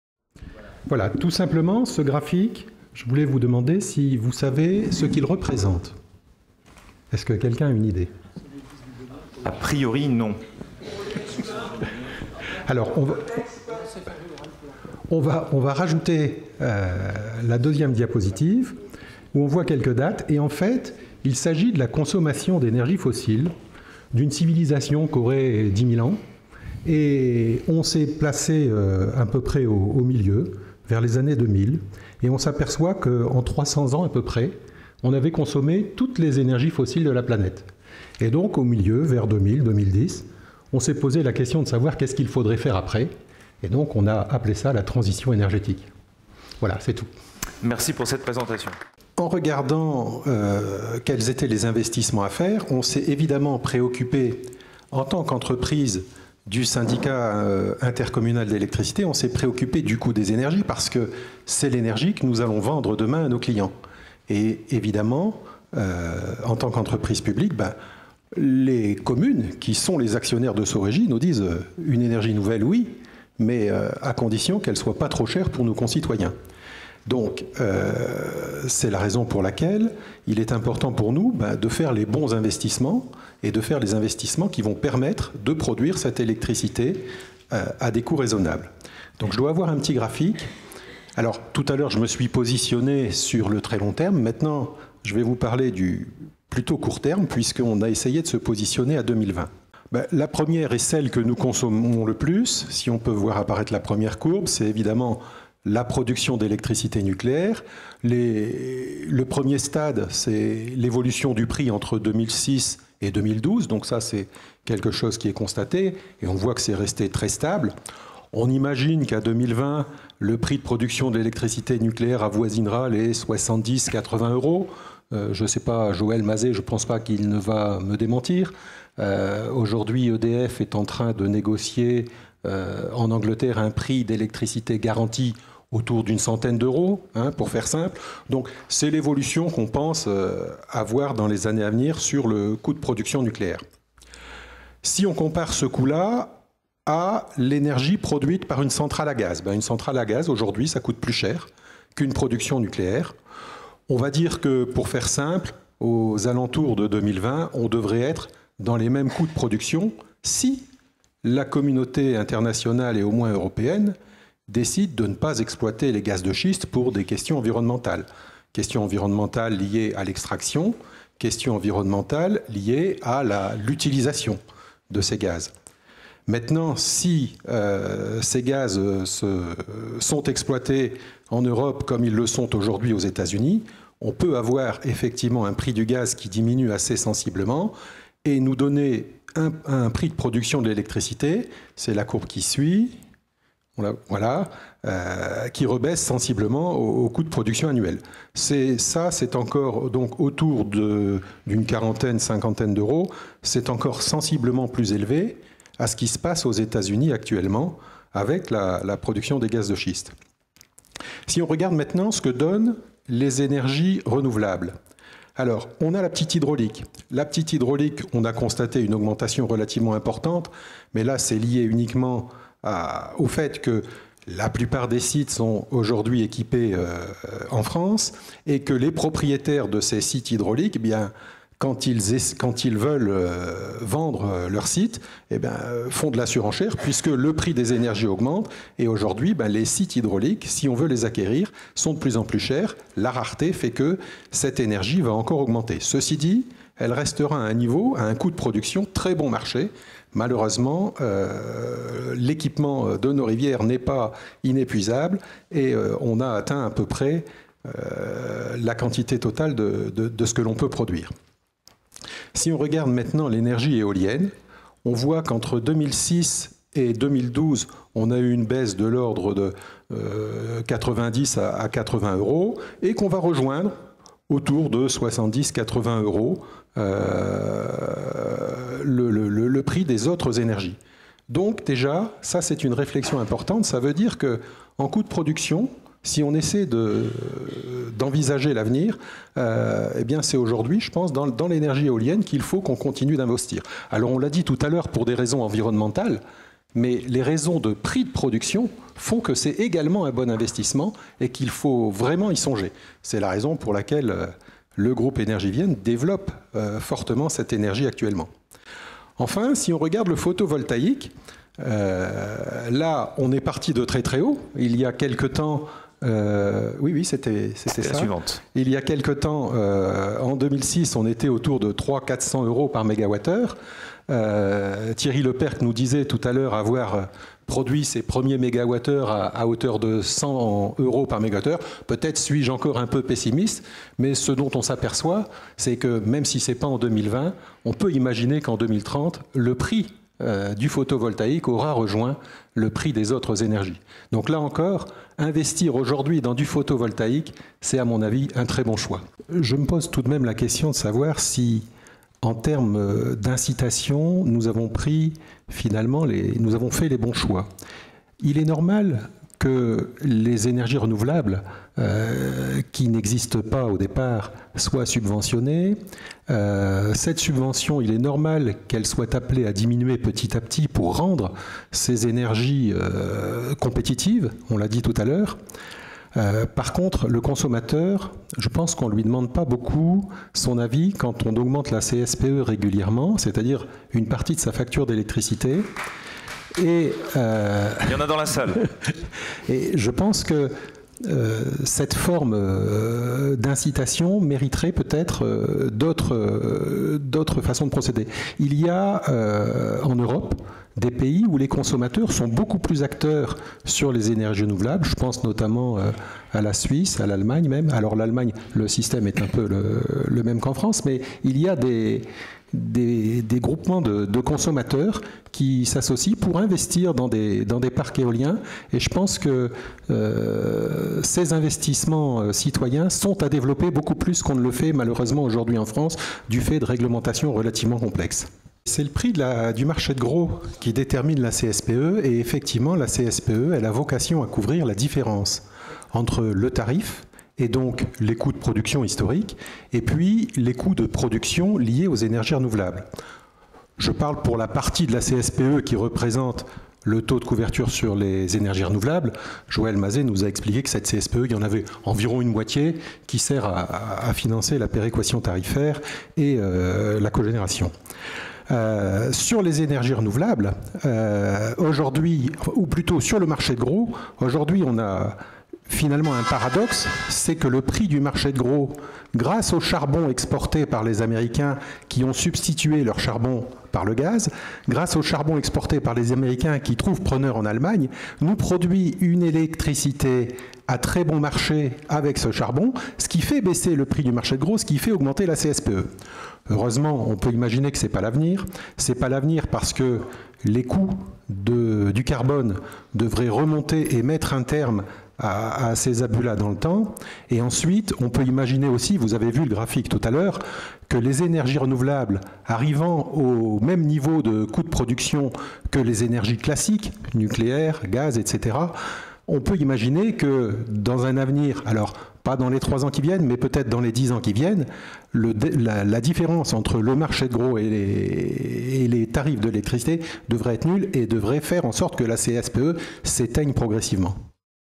Marc Loisel, Directeur de Sorégies
Intervention lors du 3e Carrefour des idées sur la transition énergétique, organisé par Jean-Michel Clément, le 17 juin 2013